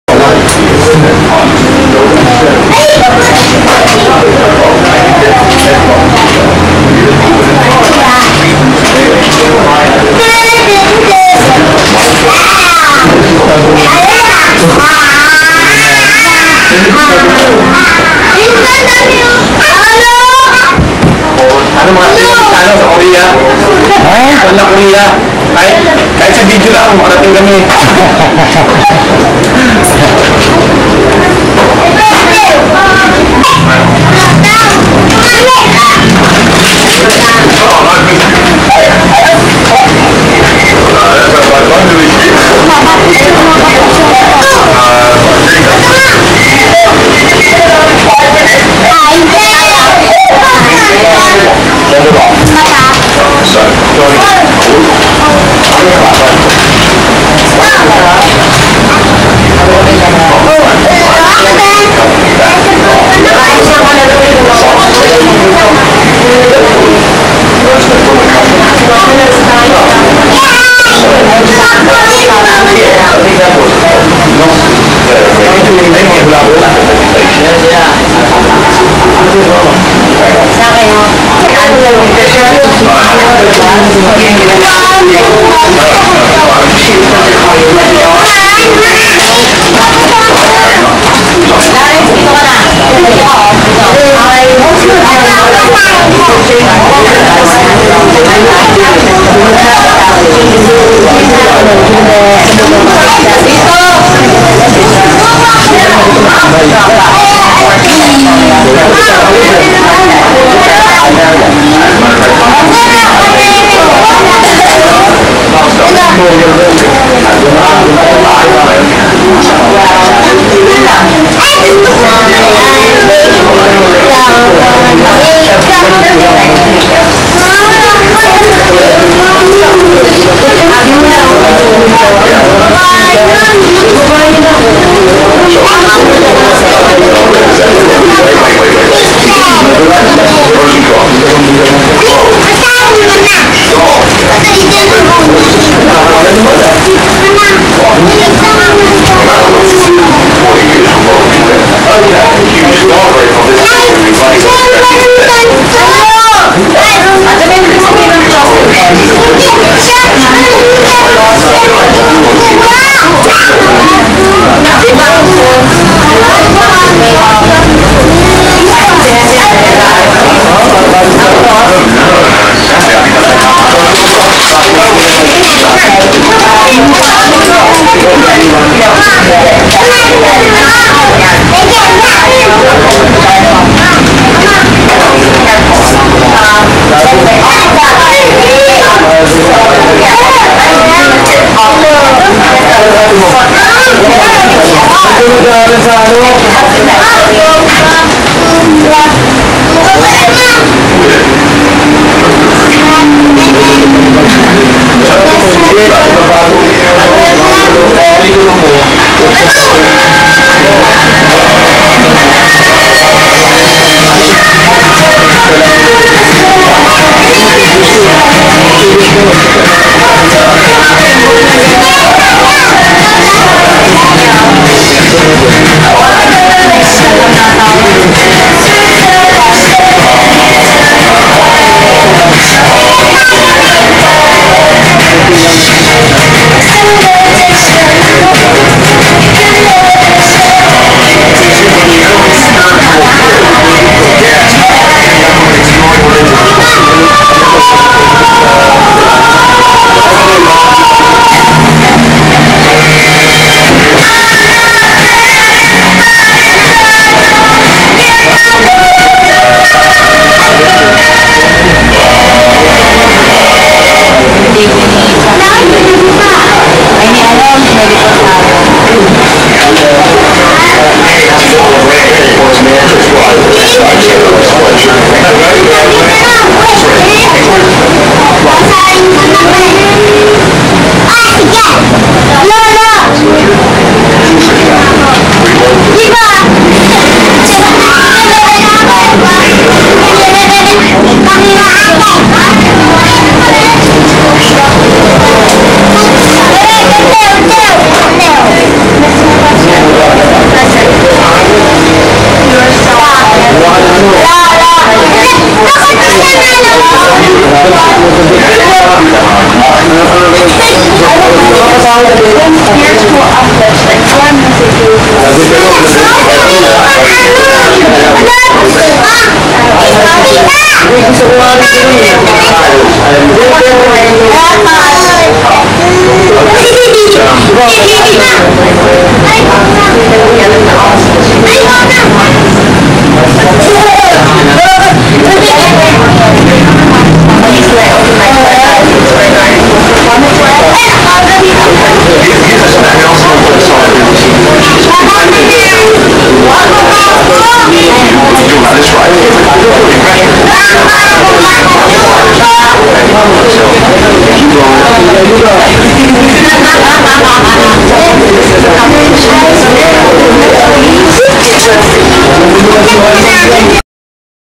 Louise 4th Birthday llllll
May 1, 2008, Louise 4th Birthday, with Pap, Ate Beth, Mommy May, Ninang Hazel, Ninong Paras
Thatin, Ninang Win, Kuya Bheng, Kuya Jhong, Kuya Knix, Thacelle, Tita CJ, Tito Lenz and
Mikee.